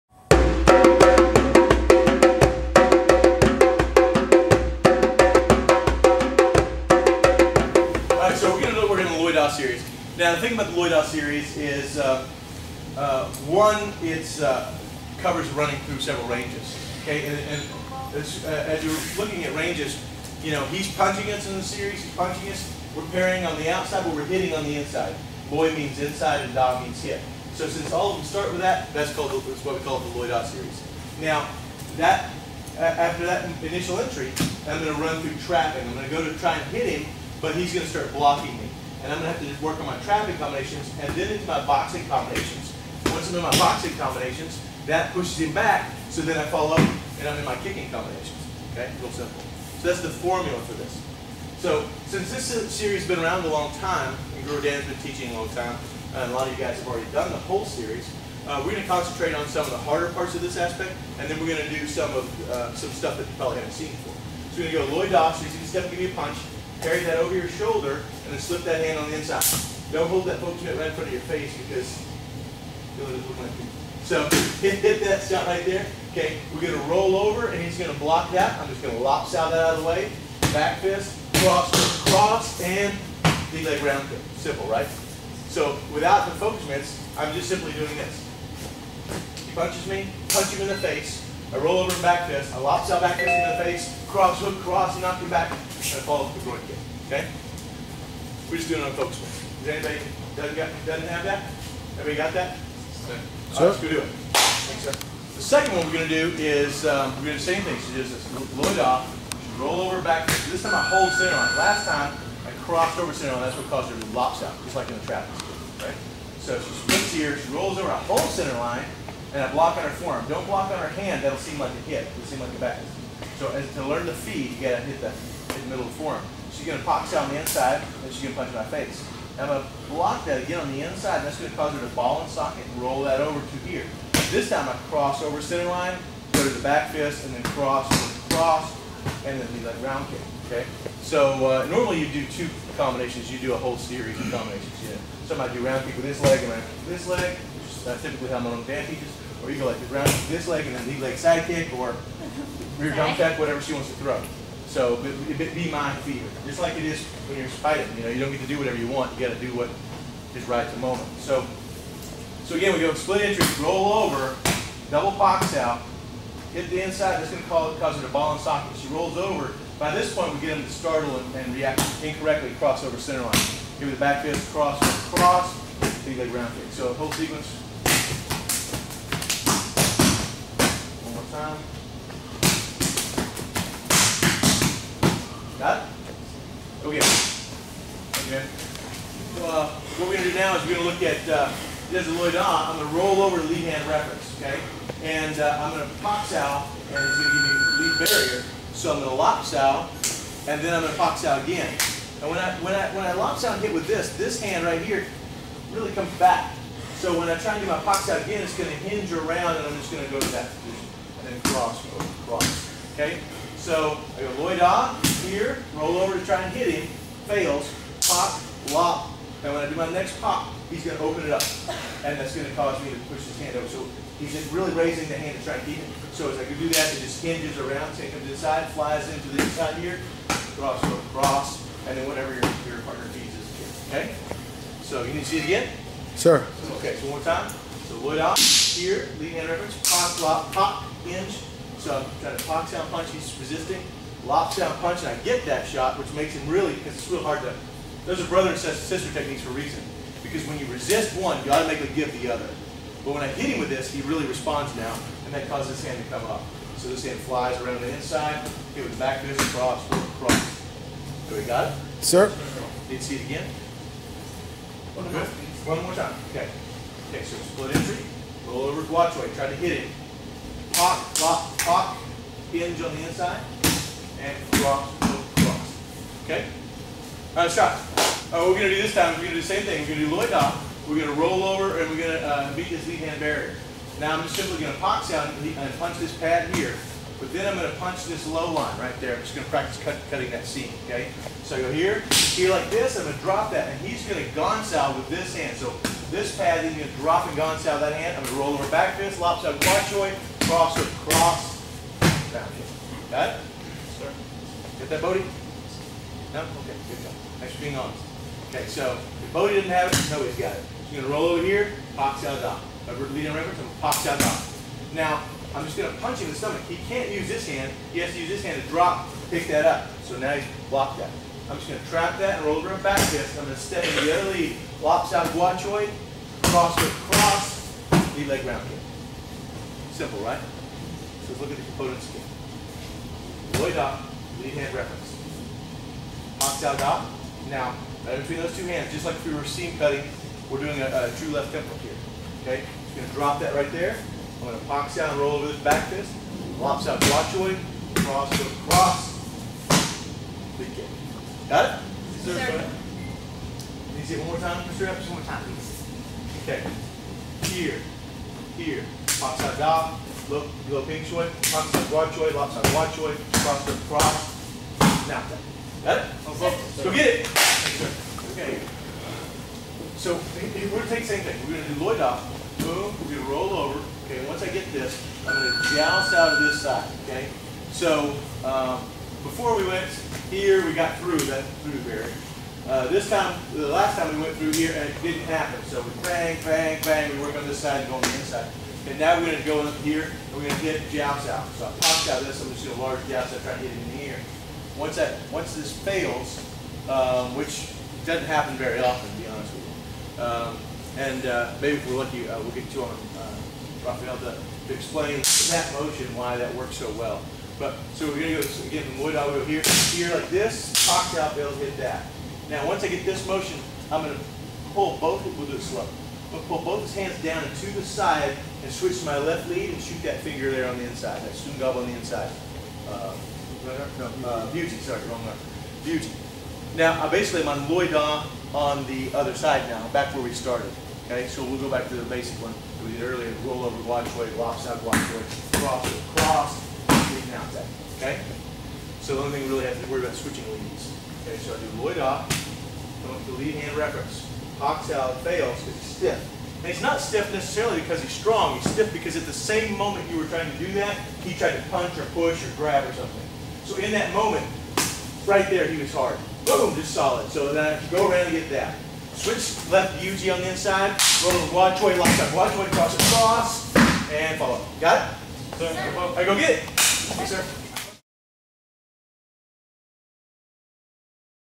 Guro Harley Elmore: Family Gathering 2011: Loy Da
Guro Harley Elmore teaching some elements from the Loy Da series.